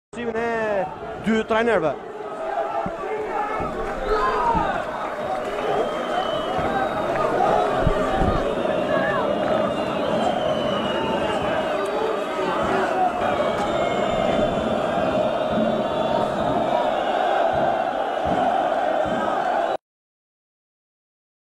Gol Liridon OSMANAJ | Teuta 0-1 PARTIZANI
Momenti i golit te Liridon OSMANAJ dhe Festimi me pas me Tifozerine,pas Perfundimit te lojes. PMTP